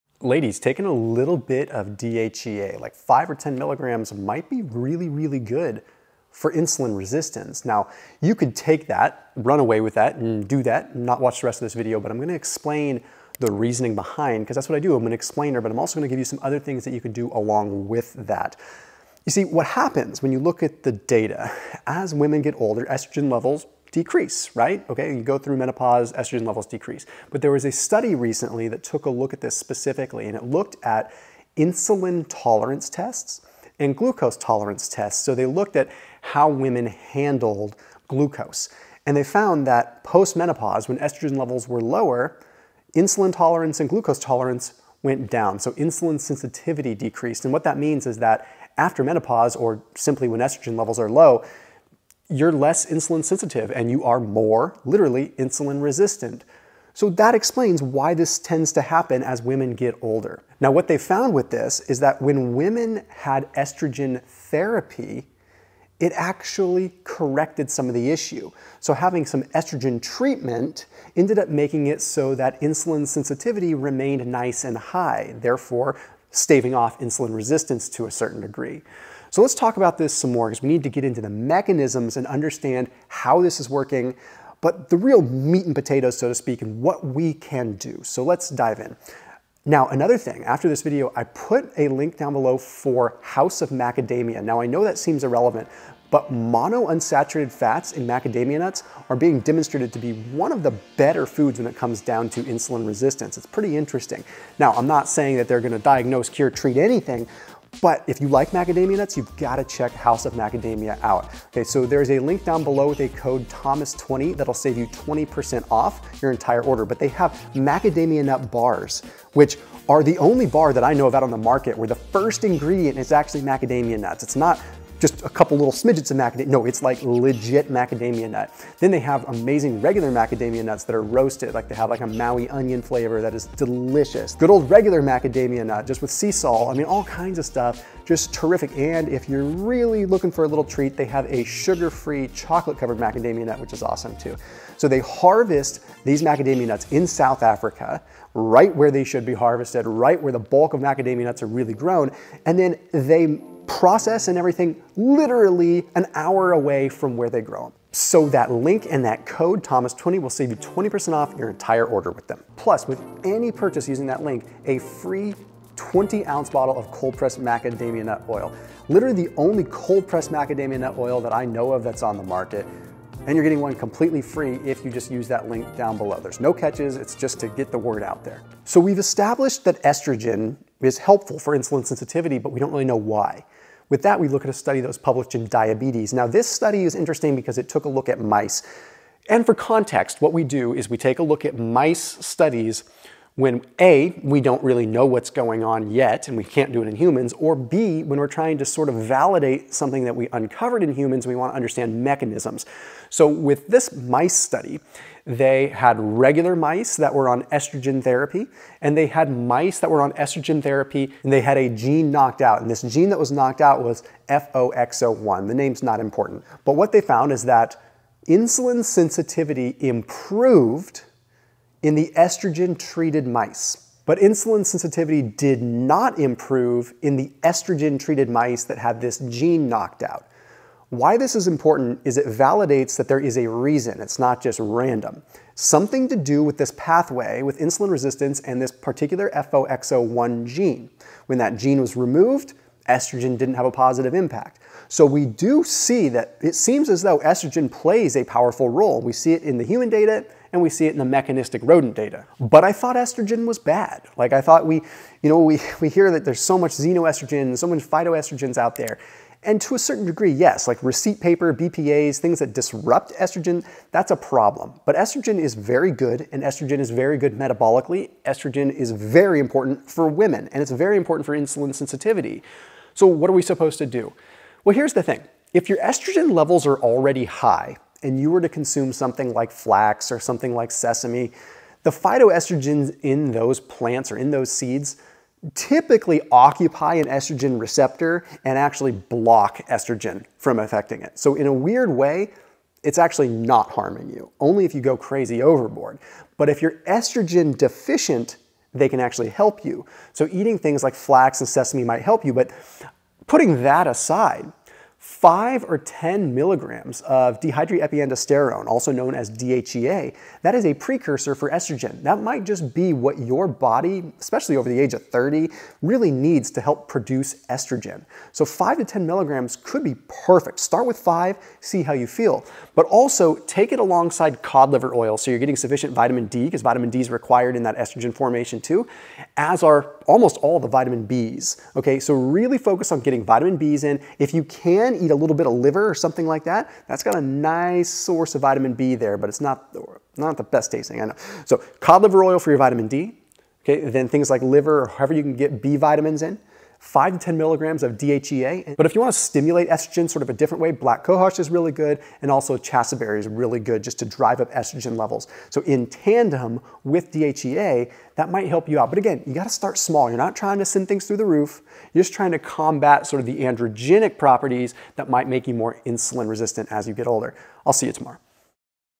DHEA Reduces Insulin Resistance in Women (and Lowers Blood Sugar)
DHEA Reduces Insulin Resistance in Women (and Lowers Blood Sugar)- Thomas DeLauer

Reference

Timestamps ⏱

0:00 - Intro - DHEA for Insulin Resistance
1:33 - Use Code THOMAS20 for 20% off House of Macadamias!
2:58 - Estrogen & Insulin Sensitivity
5:03 - DHEA